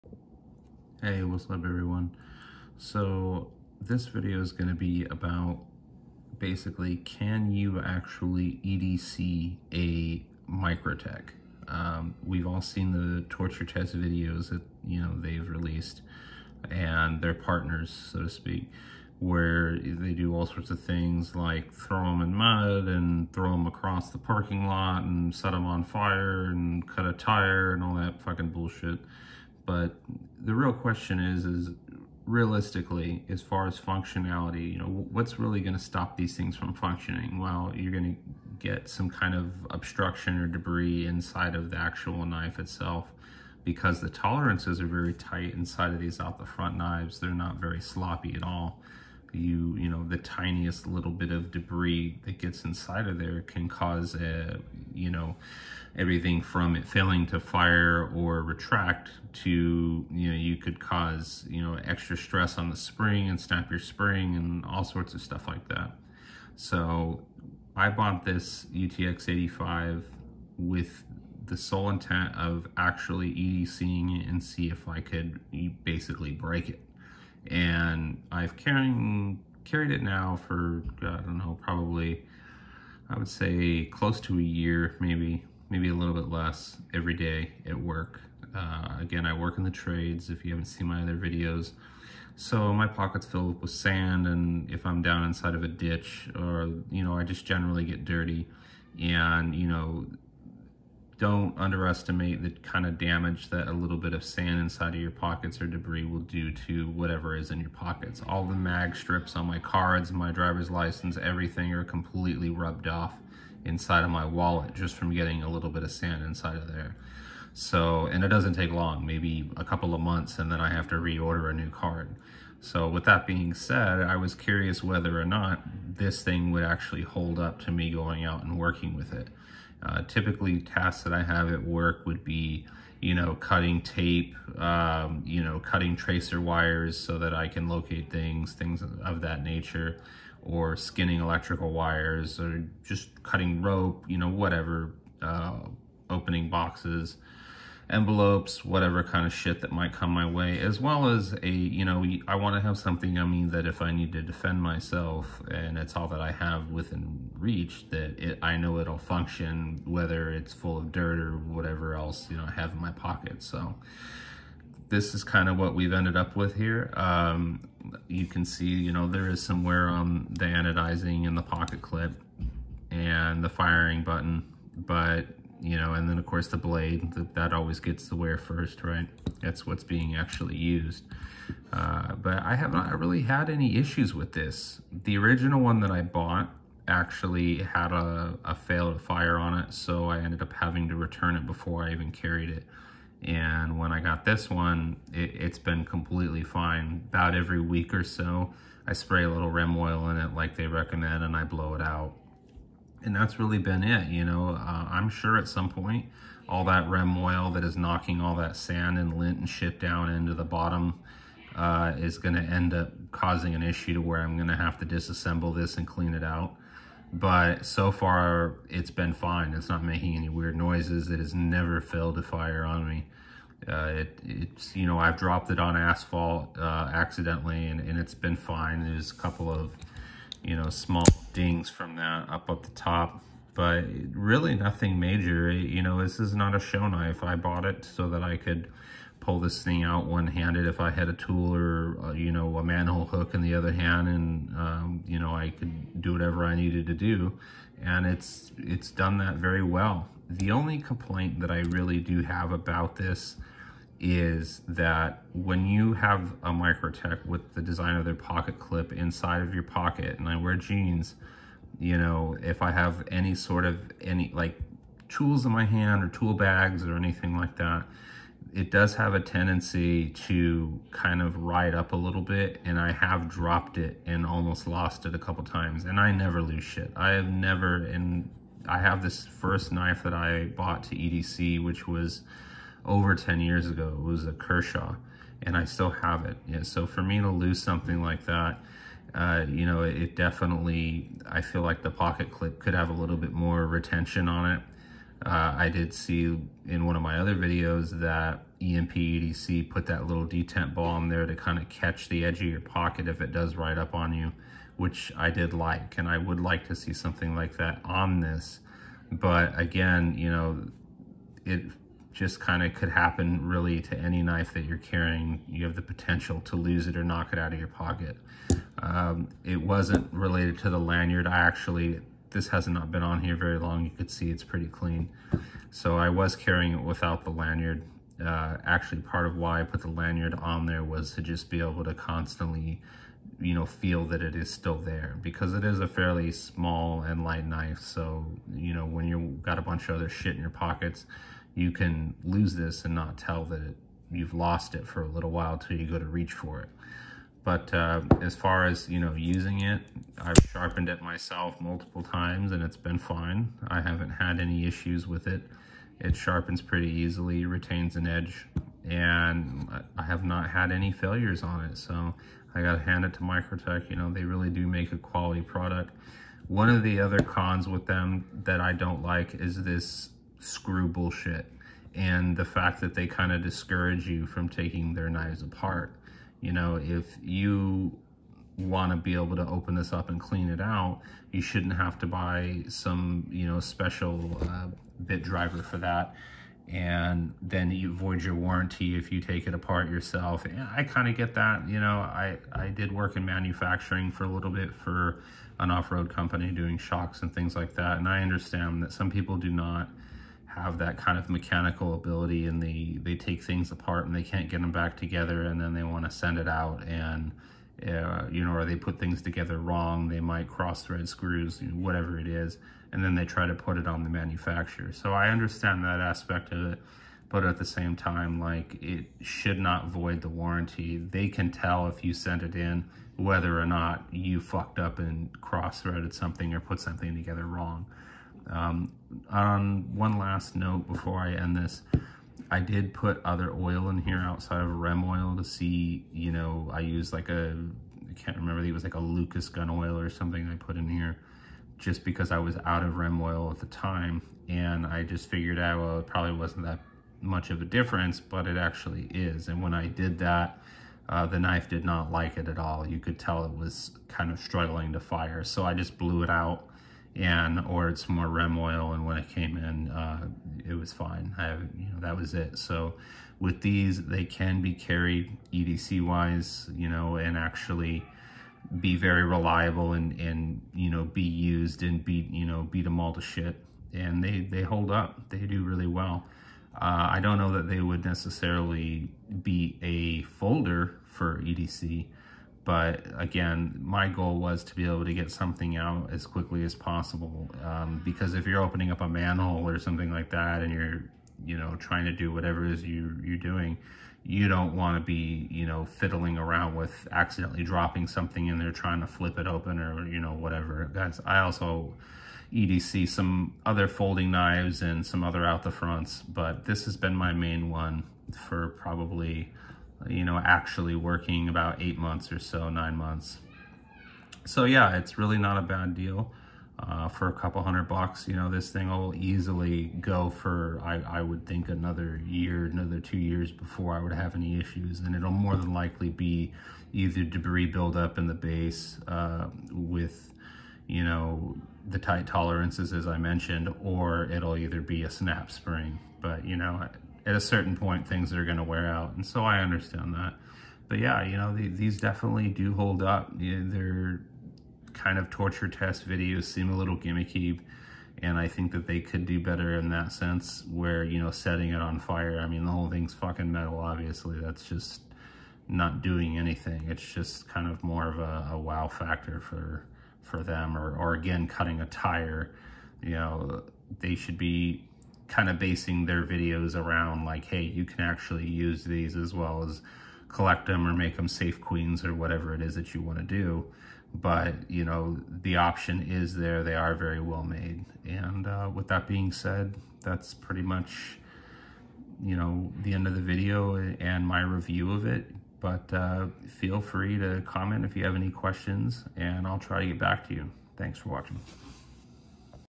Can You Really EDC A Microtech OTF?
In this video I talk about how I have been carrying a Microtech UTX-85 everyday for 8-10 months at work. I go over what I like about it and dislike, as well as a realistic take on how much of a beating these knives take. Microtech is known for quality and their torture test videos. But will it really hold up to being on a job site everyday for almost a year?